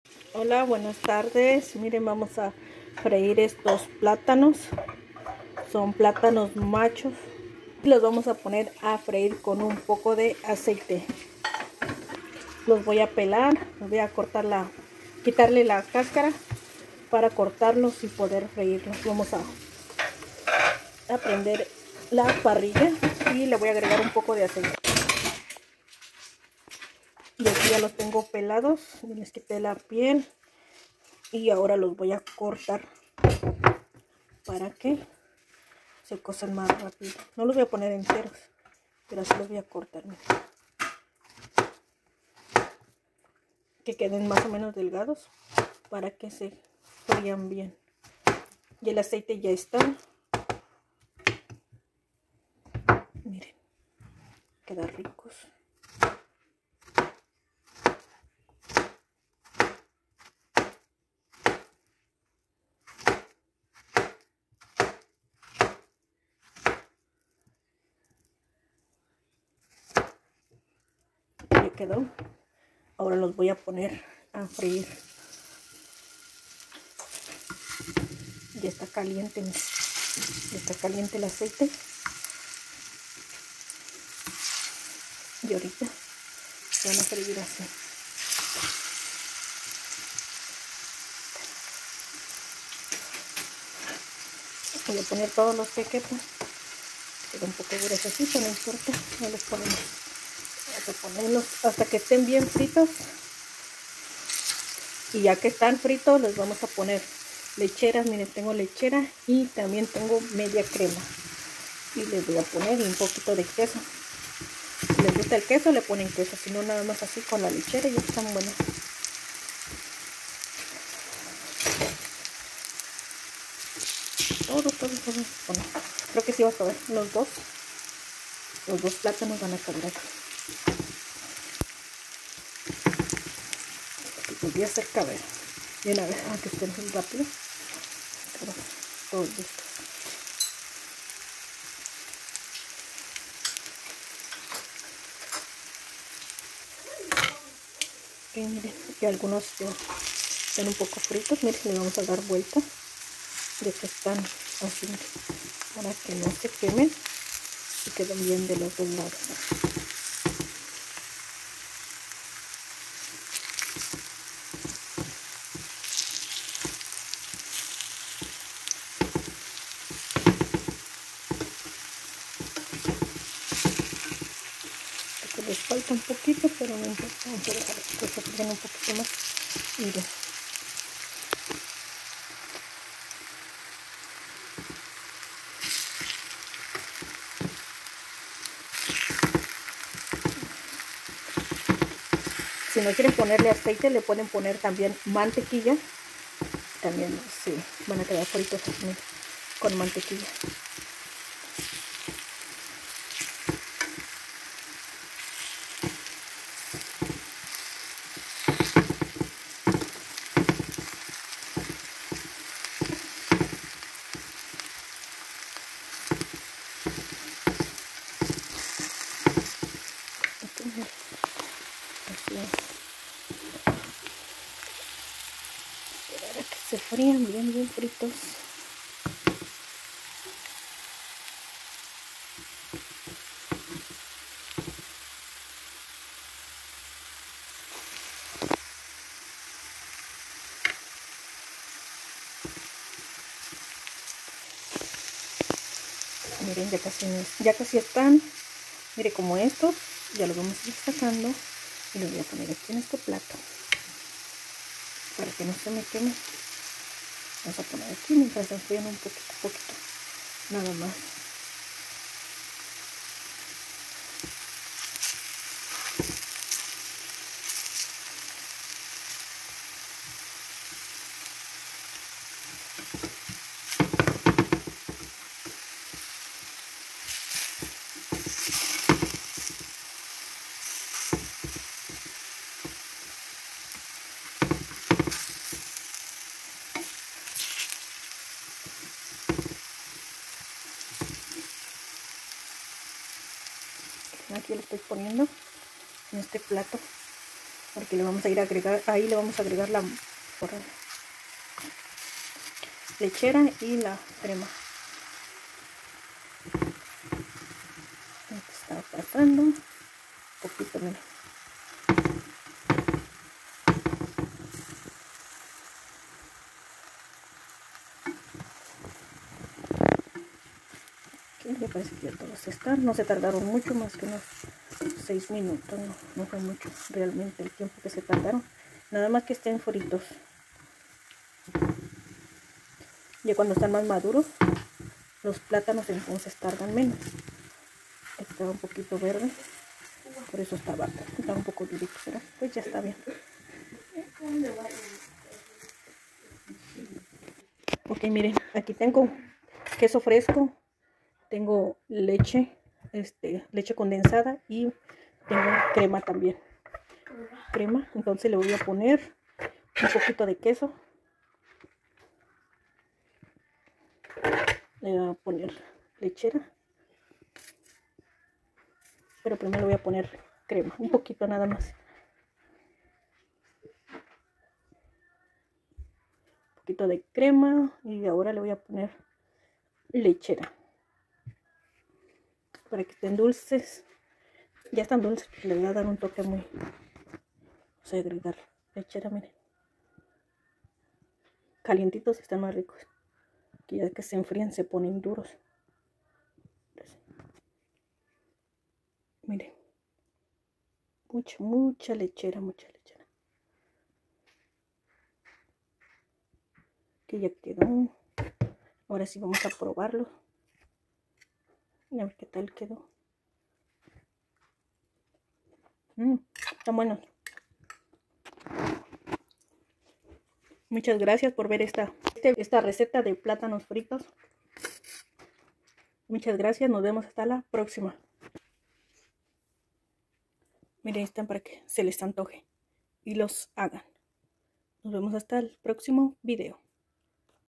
Plátanos Fritos
Ingredientes para los Plátanos Fritos
2 Plátanos Maduros ,pelados
1 .media crema
1.leche condenada
2 cucharadas queso fresco.
aceite para freír  el necesario.